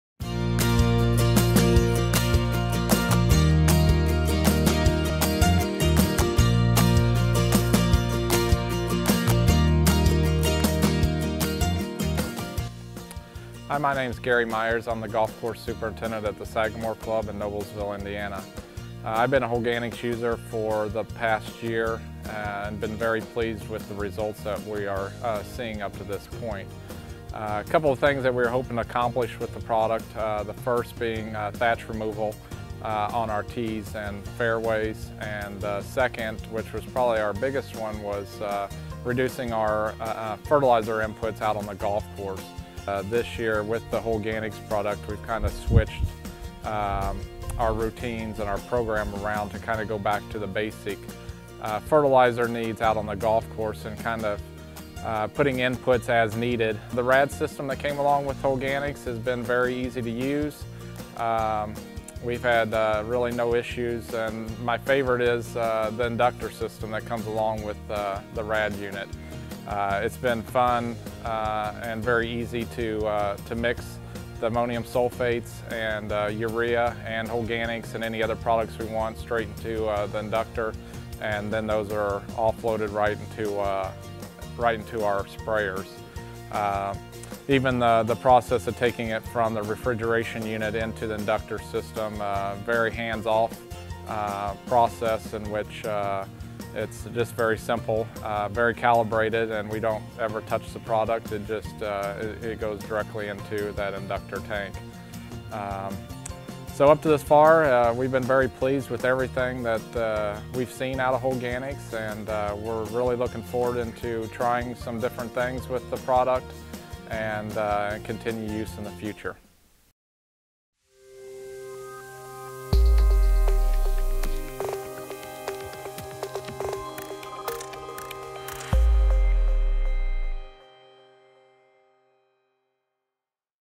Holganix Testimonial - Sagamore Golf Course
Life is good at Sagamore Golf Club, a highly revered Indiana championship course that can be compared to Muirfield Village and Medinah. This past fall, Sagamore signed on with Holganix- an organic soil amendment manufacturer to help create healthier turf with less synthetic inputs. What did Gary Myers, Superintendent of Sagamore have to say about Holganix?

Want to learn more about Sagamore's experiences with Holganix?